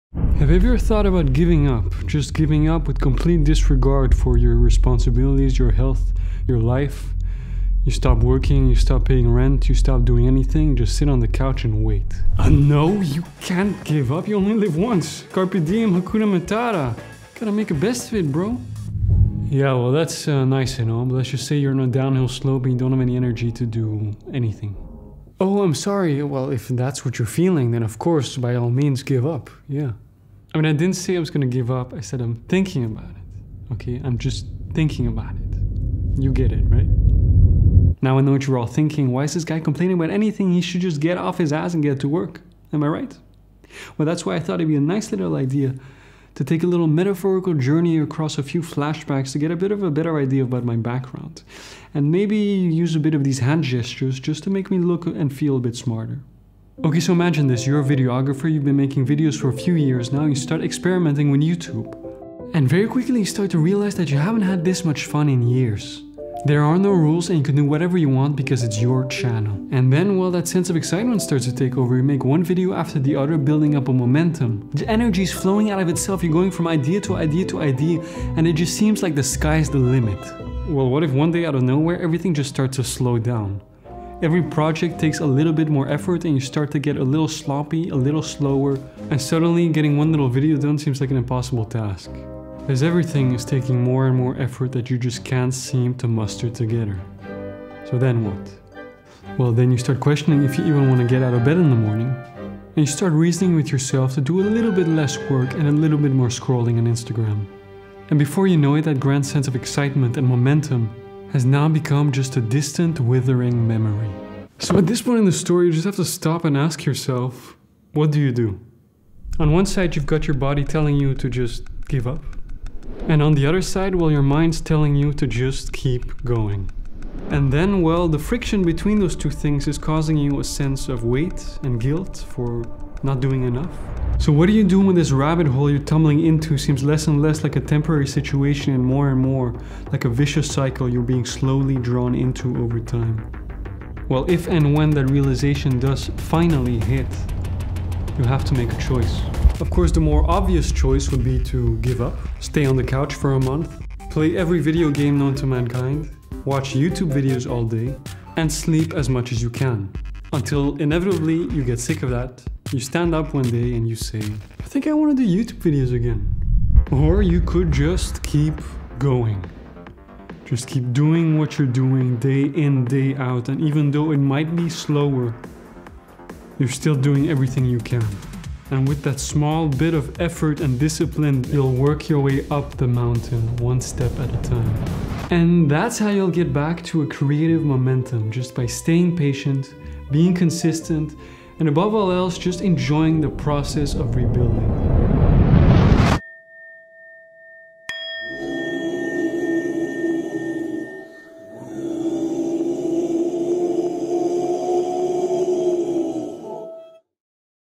This is what happens when you lose momentum
Everything is great when you're swimming in the clouds, and every step you take is creating more and more fuel for your creativity engine.

That sense of excitement is a double-edged sword I'm all too familiar with.

And it can come as quickly and spontaneously as it can leave.

But I wouldn't change it for anything in the world.

When times and moods start to pivot with the changing of seasons, I might be overcome by an initial resistance and disappointment to that very change.

I don't want to let go. Things were going so well. But that's exactly where frustration and negativity is born. In that idea of holding on.

Life is full of highs and lows; our brain is an ever-fluctuating machine of reactive tissue. Emotional consequences to the circumstances of life.

And what might feel like a low, is just another opportunity to grow, adapt, and overcome. Here's to the journey back up.